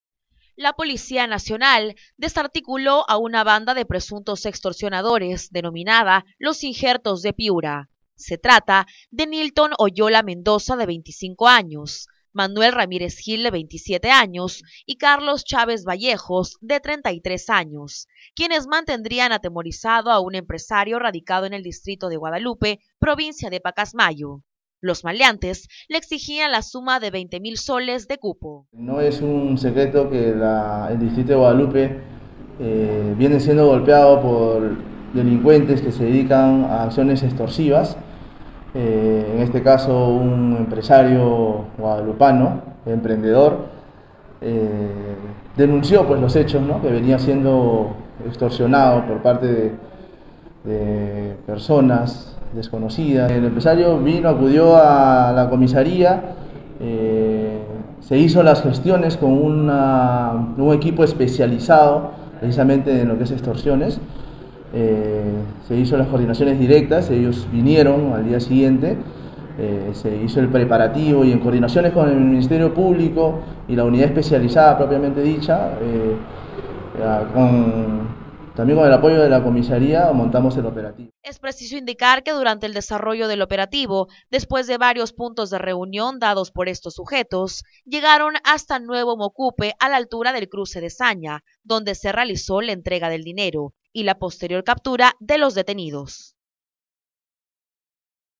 Guadalupe: “Los injertos de Piura” extorsionaban a empresario
La Policía Nacional logró la desarticulación de la banda de extorsionadores “Los Injertos de Piura” que tenían amenazado a un empresario del distrito de Guadalupe, a quien le exigían 20 mil soles a cambio de no atentar contra su vida y la de su familia.

La Policía Nacional desarticuló a una banda de presuntos extorsionadores denominada “Los Injertos de Piura”. Se trata de Nilton Oyola Mendoza de 25, Manuel Ramírez Gil de (27) y Carlos Chávez Vallejos de (33), quienes mantendrían atemorizado a un empresario radicado en el distrito de Guadalupe, provincia de Pacasmayo. Los maleantes le exigían s/20 mil de cupo.

Es preciso indicar que, durante el desarrollo del operativo después de varios puntos de reunión dados por estos sujetos, llegaron hasta nuevo Mocupe a la altura del Cruce de Zaña, donde se realizó la entrega del dinero y posterior captura de los detenidos.